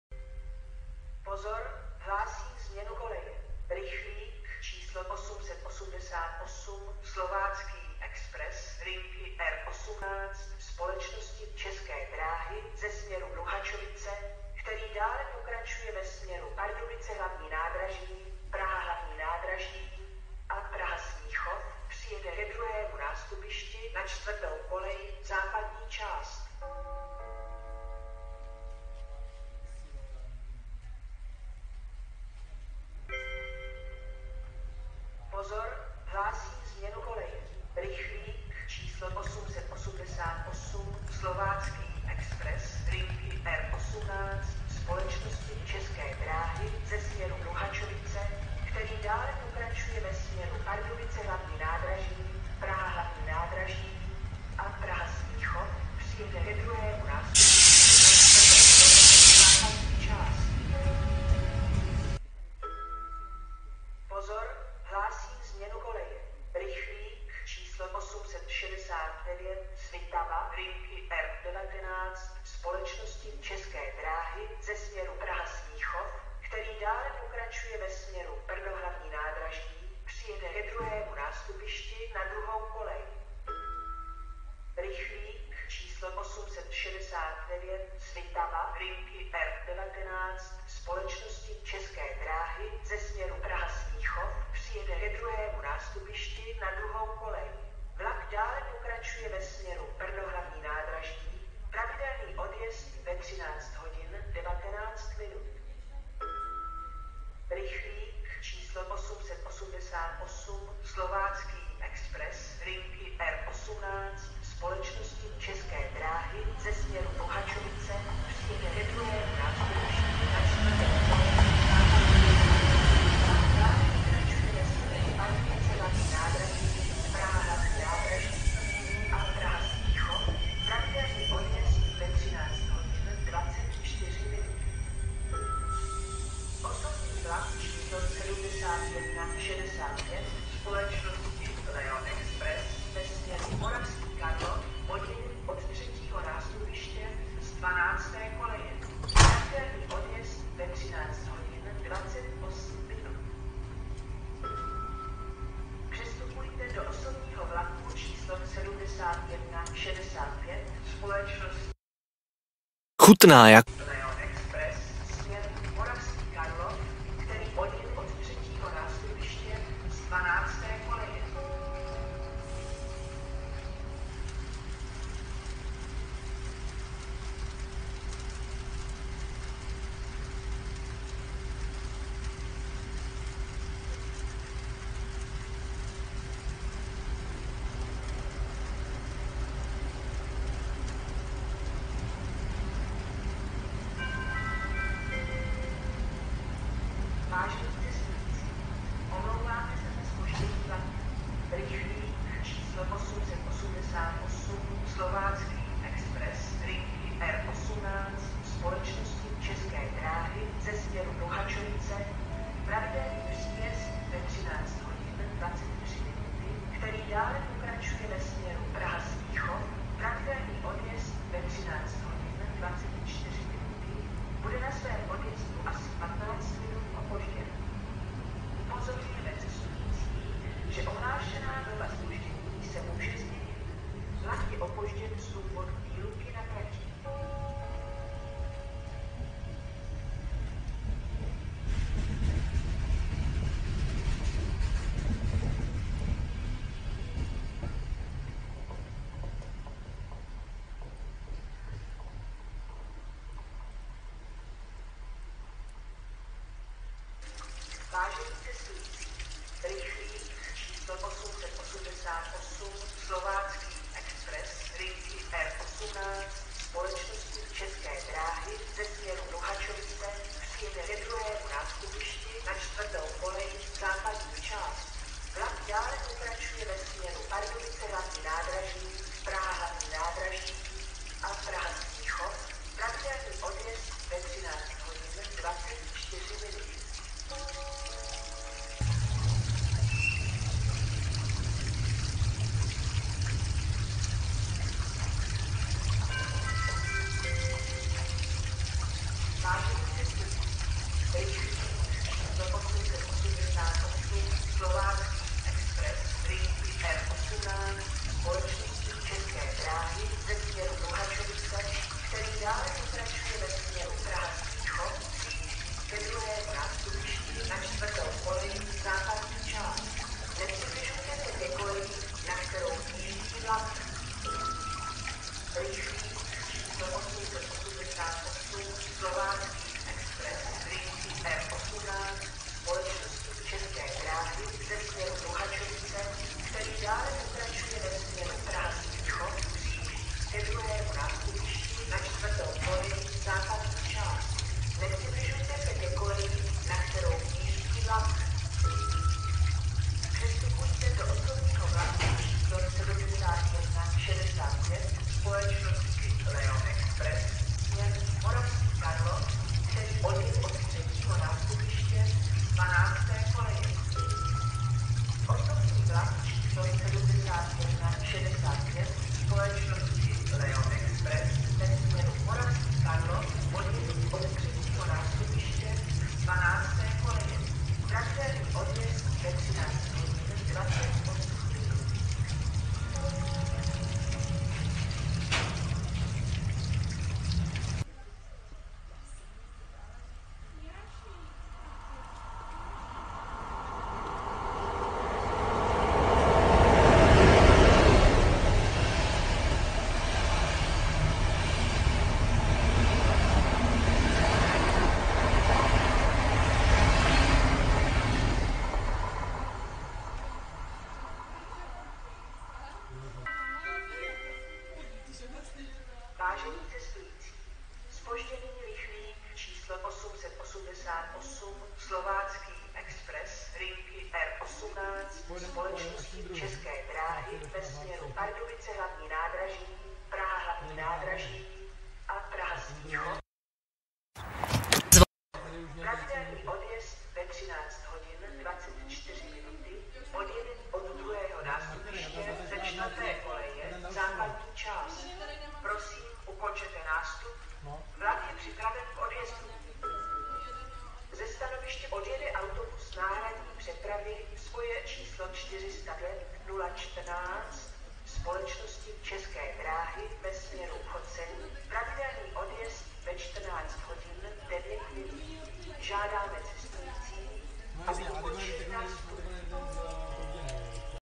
Staniční hlášení a vlak Ústí nad Orlicí - R 888 + R 869+změna koleje + Os 7165 + Zpoždění, 30.8.2021
0:00 R 888 Slovácký expres ( změna koleje )
0:33 R 888 Slovácký expres ( změna koleje ) + Průjezd vlaku
1:02 R 869 Svitava ( změna koleje )
1:23 R 869 Svitava ( přijede )
1:46 R 888 Slovácký expres ( přijede ) + Příjezd vlaku R 869 Svitava
2:15 Os 7165 Leo Express ( odjede )
2:34 Os 7165 Leo Express ( přestupujte )
2:54 Průjezd vlaku + R 888 Slovácký expres ( bude opožděn +15 min ) + Odjezd vlaku R 869 Svitava
4:17 R 888 Slovácký expres ( přijede )
4:49 Příjezd vlaku R 888 Slovácký expres + R 888 Slovácký expres ( přijíždí )
5:24 R 888 Slovácký expres ( přijíždí )
5:48 Os 7165 Leo Express ( přestupujte )
6:03 Os 7165 Leo Express ( odjede )
6:38 Průjezd vlaku Regiojet
6:53 Zpožděný R 888 Slovácký expres ( ukončete nástup )
7:29 Autobus náhradní přepravy spoje číslo 405014 ( odjede, ukončete nástup )
PS: Z Důvodu zastavení provozu mezi stanicemi Ústí nad Orlicí - Brandýs nad Orlicí byl R 888 opožděn na odjezdu ze stanice.